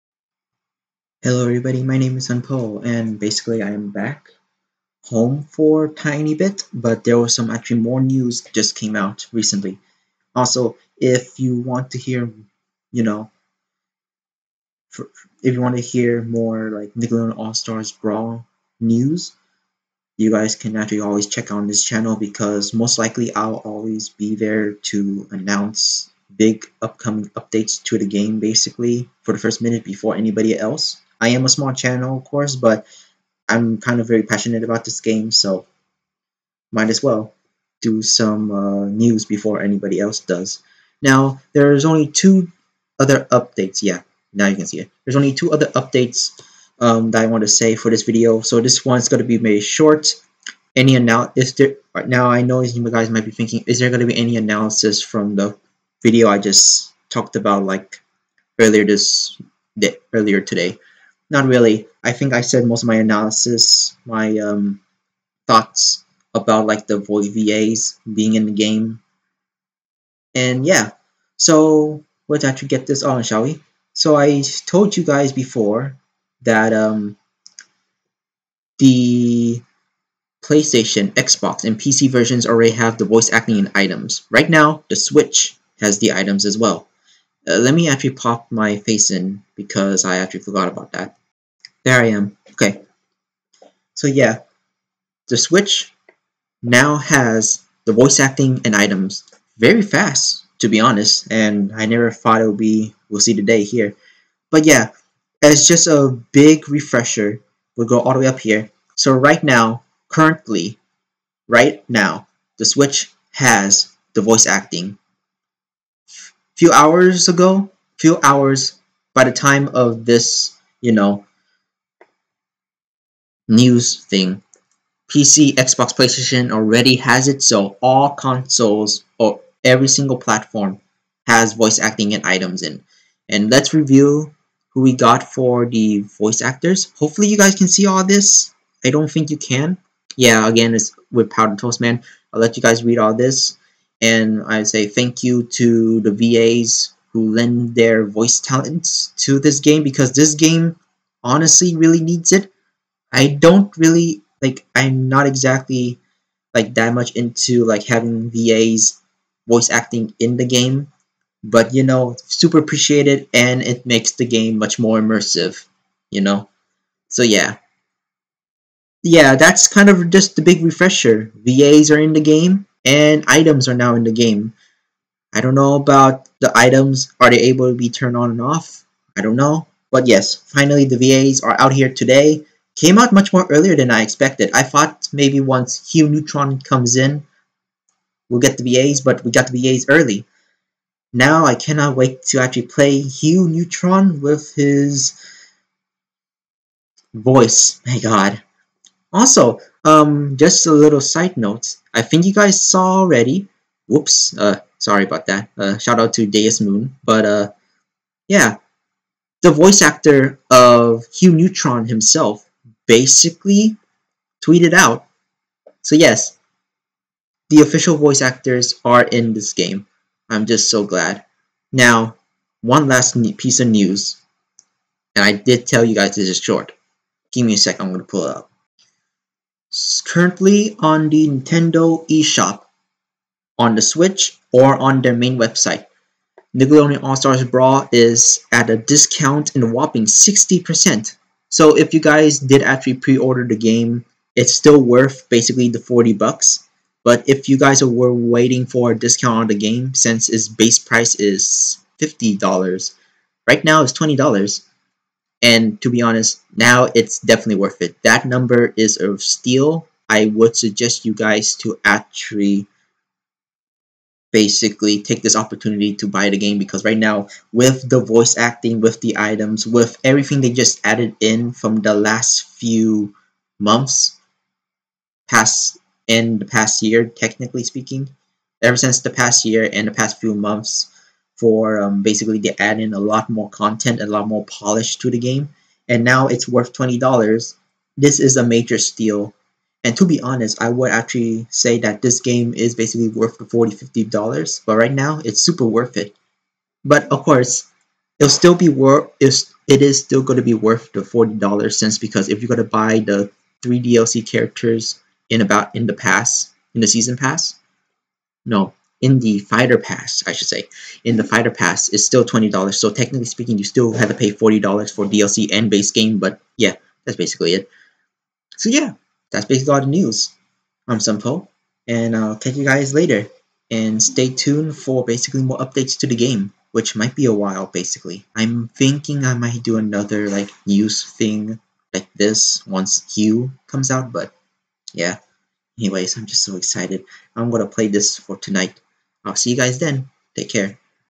Nickelodeon All Star Brawl is $60 off in the Nintendo eshop! + Voice acting is now on all versions!
The final piece of news for now! And by Hugh Neutron VA announcing it too as well!?
Also, the game is on sale for now!